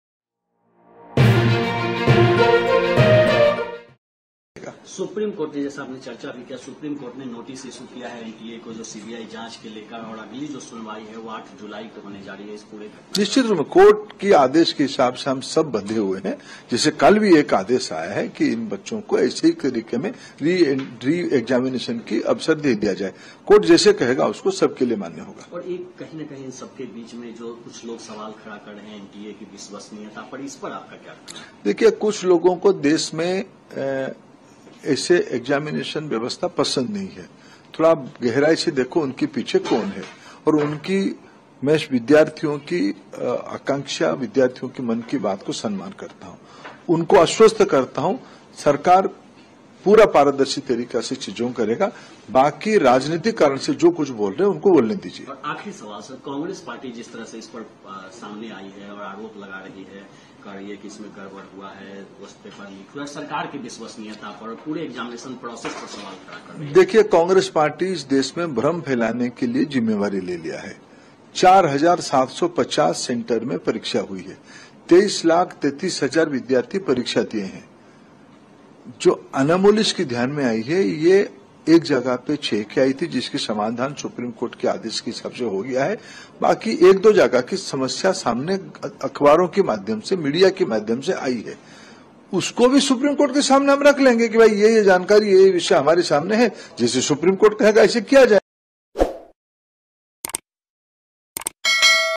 NEET : केन्द्रीय शिक्षा मंत्री बोले, "कांग्रेस पार्टी ने इस देश में भ्रम फैलाने की जिम्मेदारी ली है"
NEET परीक्षा पर सियासत: केन्द्रीय शिक्षा मंत्री धर्मेन्द्र प्रधान बोले, "...कांग्रेस पार्टी ने इस देश में भ्रम फैलाने की जिम्मेदारी ले ली है..

केन्द्रीय शिक्षा मंत्री धर्मेन्द्र प्रधान ने कहा कि कुछ विद्यार्थी आज मुझसे मिलना चाहे मैंने उनको बुलाया। मैंने उनके पक्ष को भी सुना। मैंने उनको आश्वस्त किया। सरकार सारे विद्यार्थियों के मन में इस निश्चितता के लिए प्रतिबद्ध है कि पारदर्शी प्रक्रिया अपनाई जाएगी। 23 लाख से अधिक बच्चों ने परीक्षा दी है। ग्रेस नंबर की आपत्ति का समाधान किया गया है... NEET को लेकर 41 याचिकाएं पड़ी हैं। ये सारा विषय कोर्ट की संज्ञान में है। हमारे पास जितनी भी जानकारी रहेगी हम वे कोर्ट के सामने रखेंगे। धर्मेन्द्र प्रधान ने कहा कि कांग्रेस पार्टी ने इस देश में भ्रम फैलाने की जिम्मेदारी ले ली है। 4,750 सेंटर में परीक्षा हुई है। 23 लाख बच्चों ने परीक्षा दी है... अखबारों के माध्यम से 1-2 जगह की समस्या आई है हम उसको भी सुप्रीम कोर्ट के सामने रख लेंगे। जो सुप्रीम कोर्ट कहेगा वैसा किया जाएगा